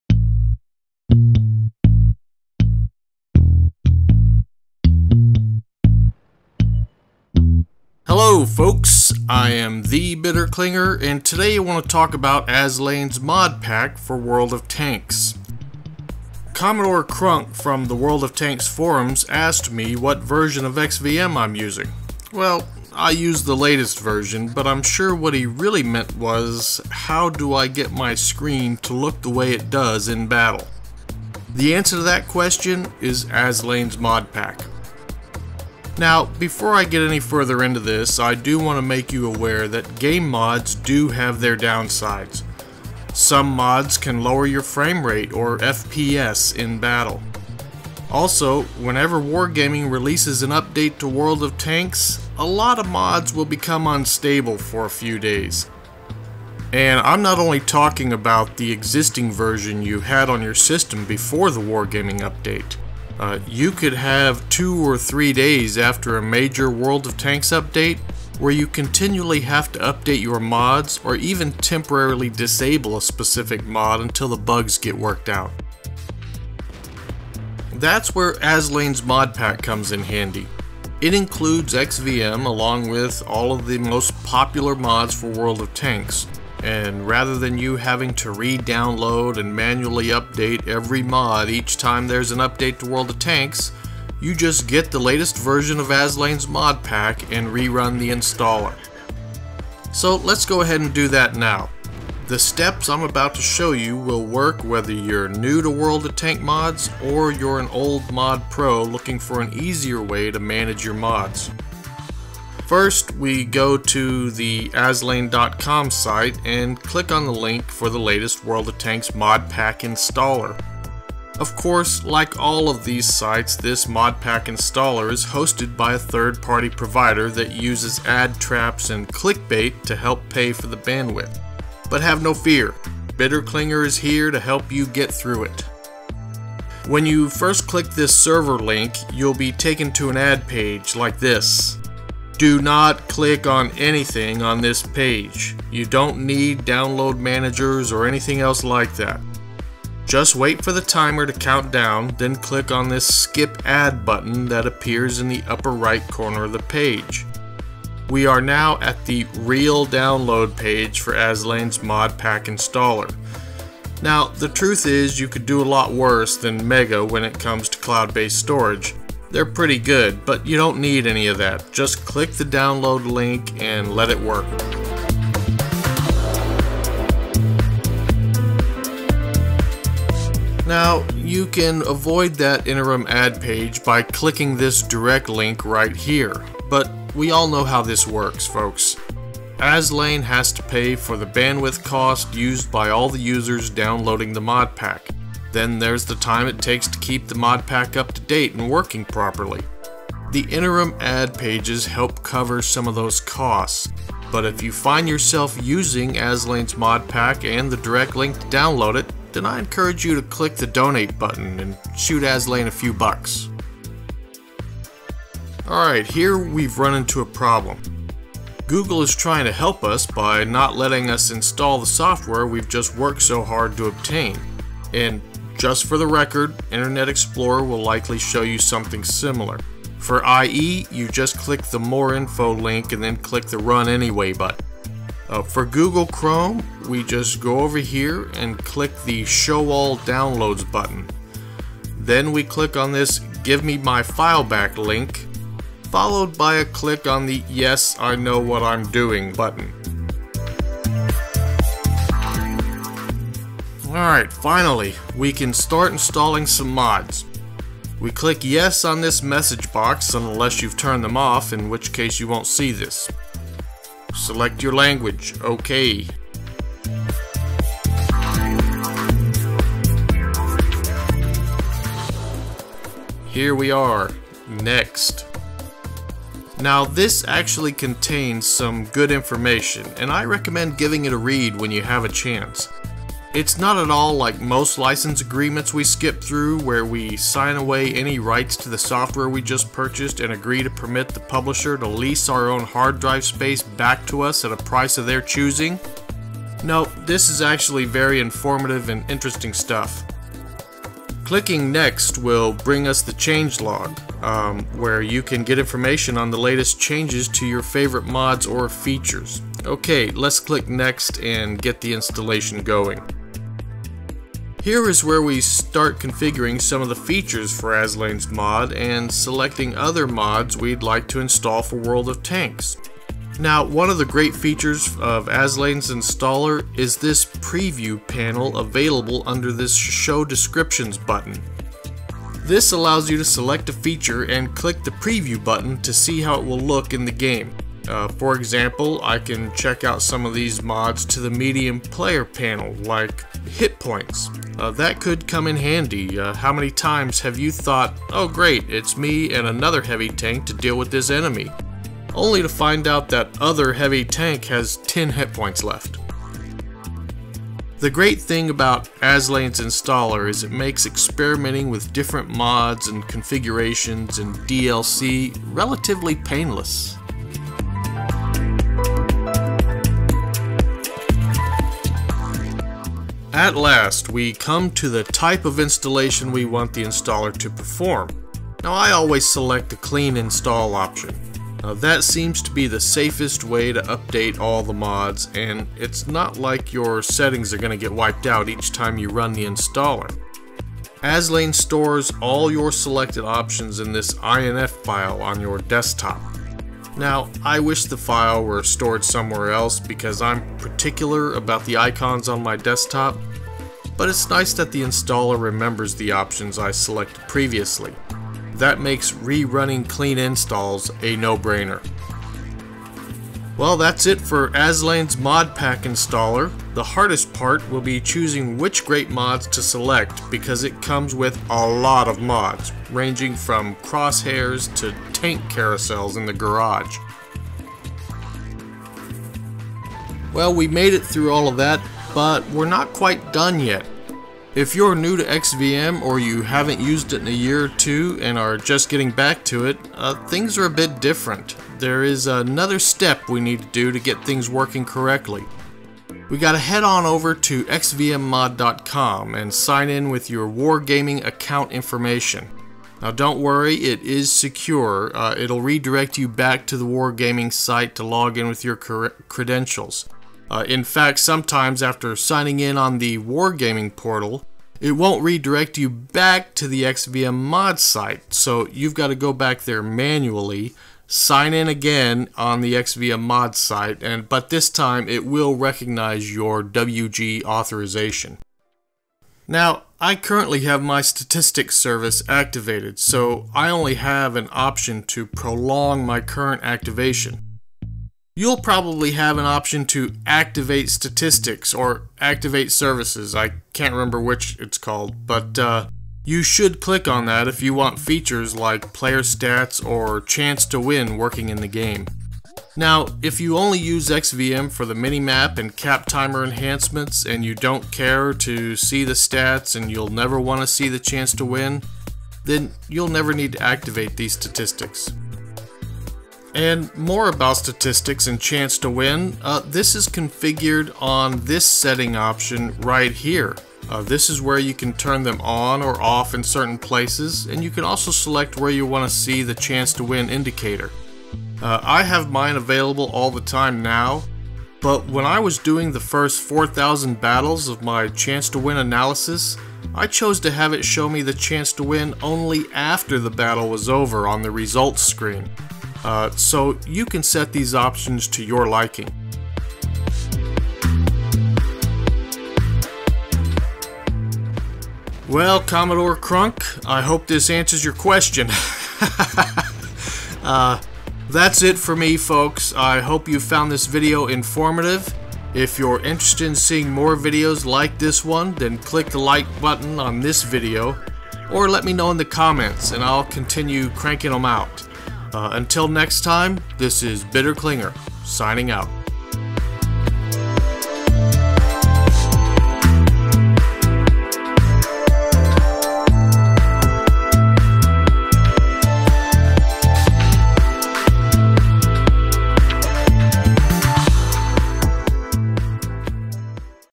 How To: XVM and Aslain's Mod Pack Installer
This video is a step by step walk-through and explanation of downloading Aslain's ModPack Installer and Activating Statistics for the XVM Mod.